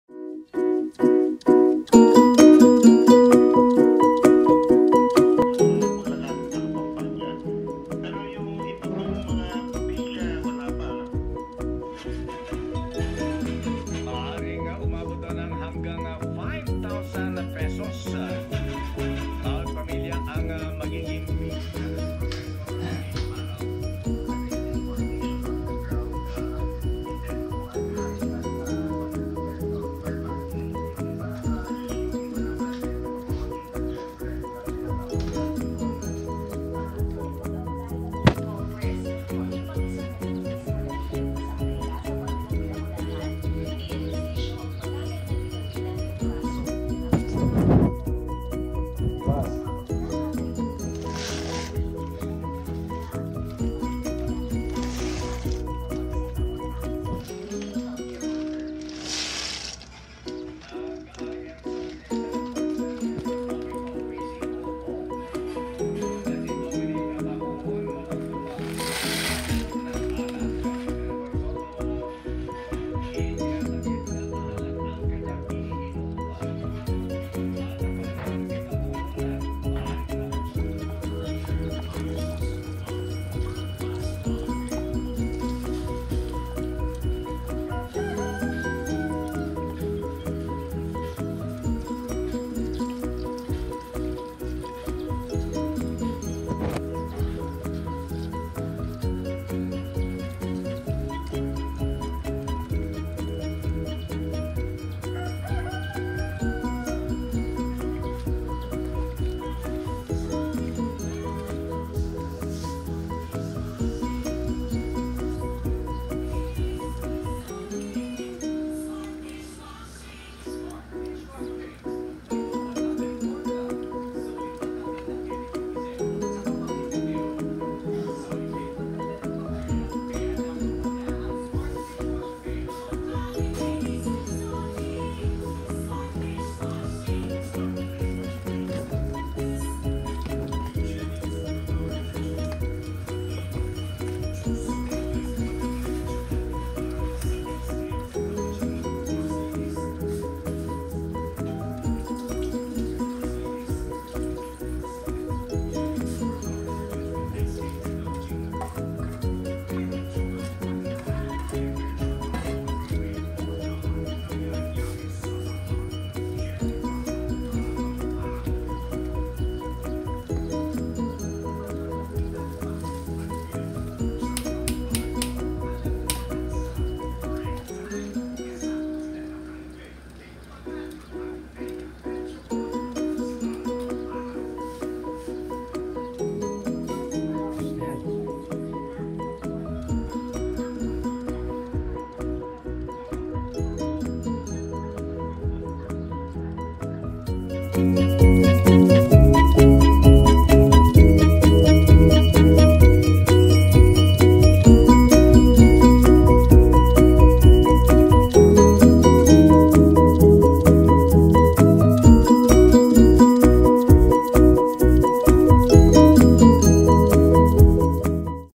Teody’s MENU: SOPAS
Sabado, walang online classes. Mainit na sabaw, pangtapos ng module. Pagkatapos, Pa-relax-relax.